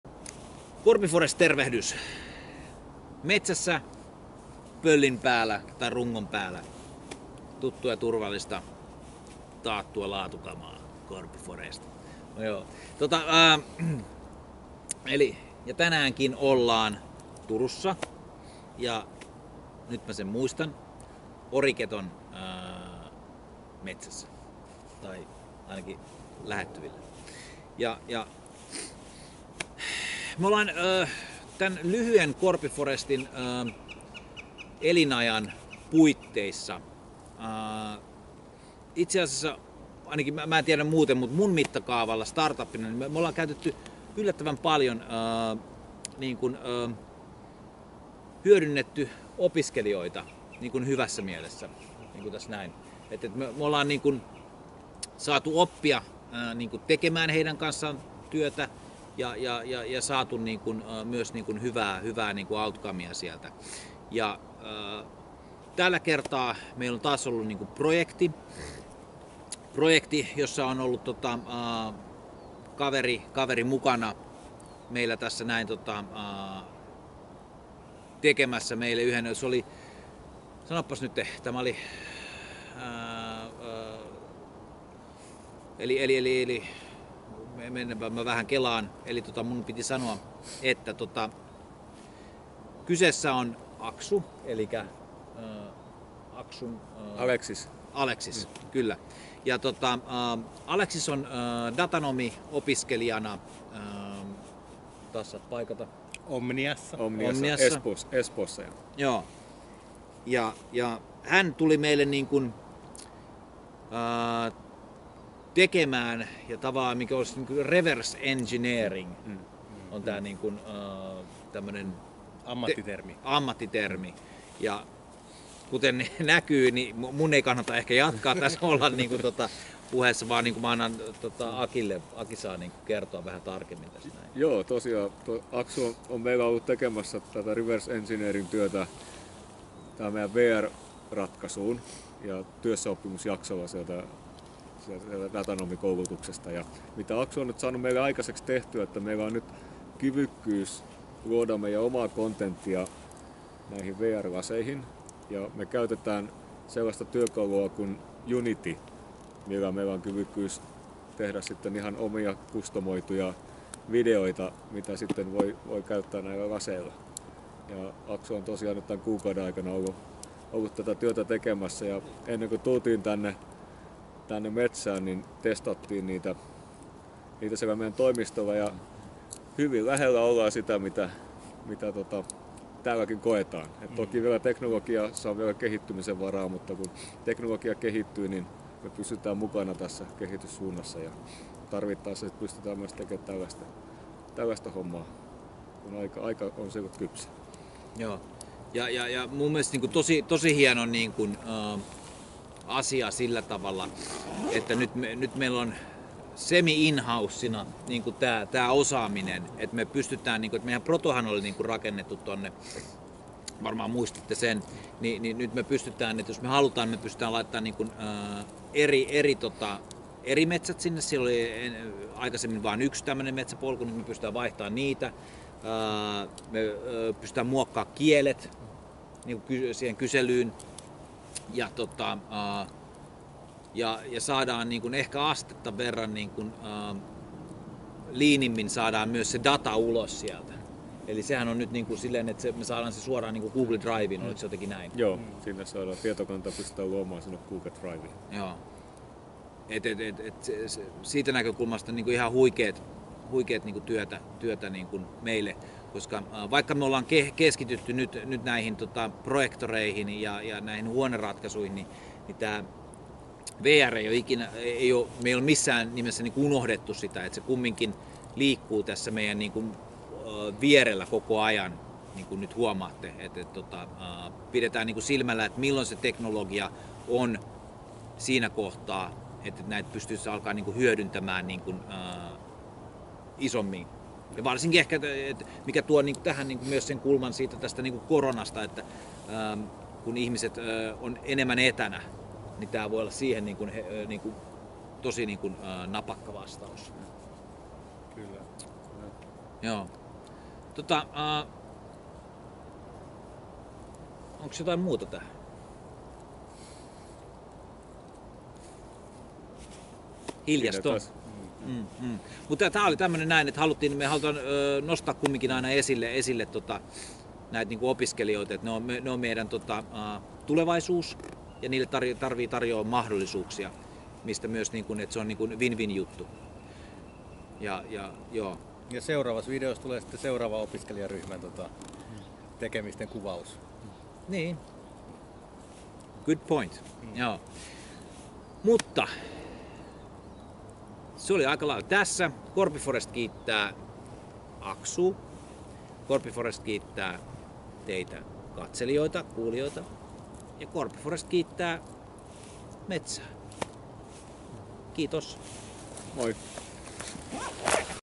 Korpi ForRest huhtikuun 2020 ihminen
Joka kuukausi valitsemme matkallamme tärkeän henkilön kuukauden ihmiseksi osoituksena arvostuksesta ja kiitoksesta. Tällä kertaa kyse on opiskelijasta. Katso, kenet valitsimme!